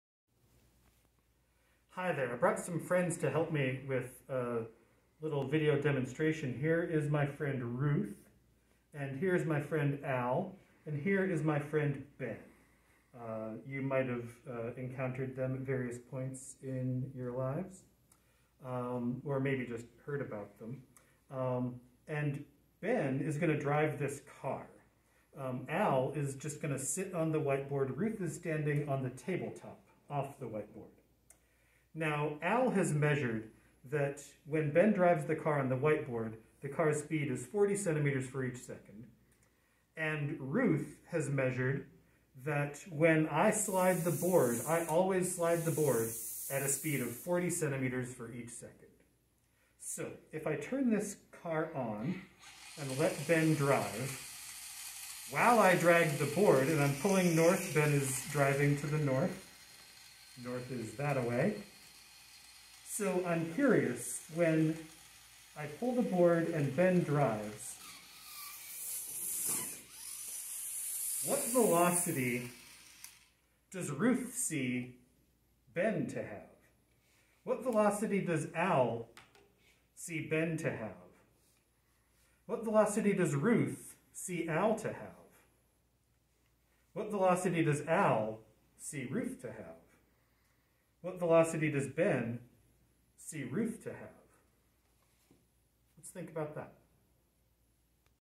Relative Motion 1
How will the velocities of Ben, Al, and Ruth compare measured by each person?